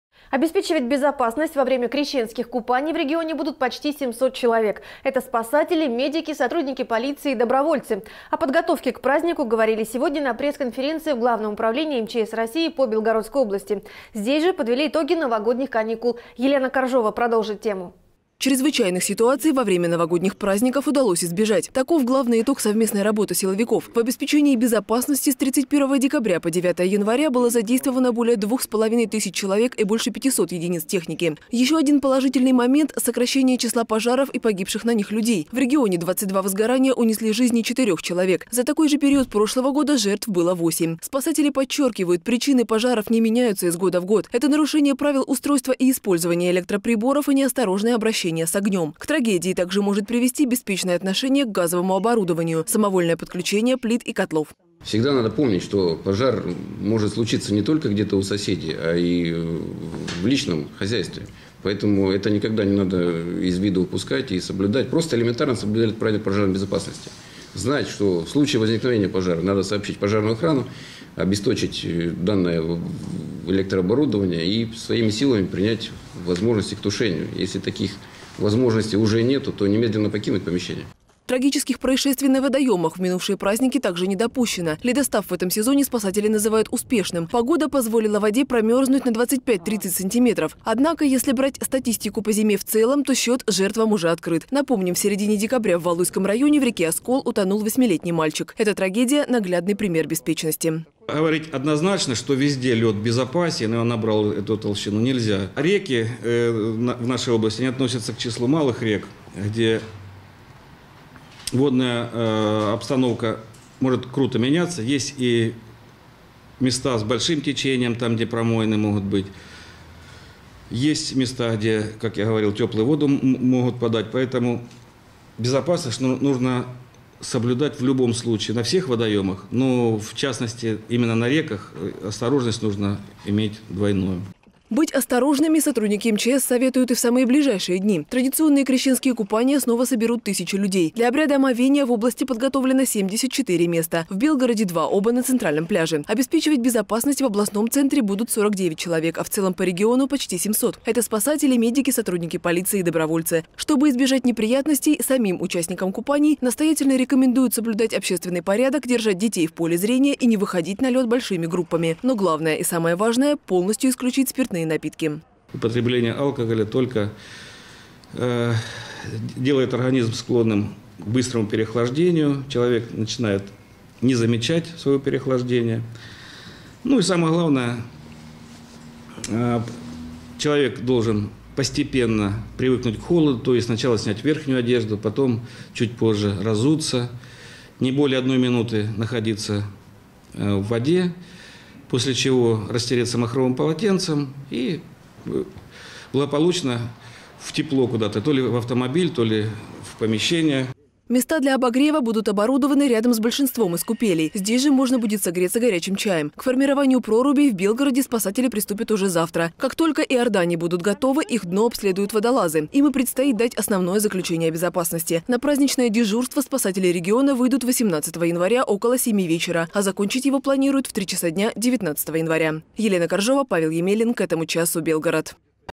Контролировать безопасность на Крещение в Белгороде будут 49 человек
Обеспечивать безопасность верующих во время крещенских купаний в регионе будут почти семьсот человек — это спасатели, медики, сотрудники полиции и добровольцы.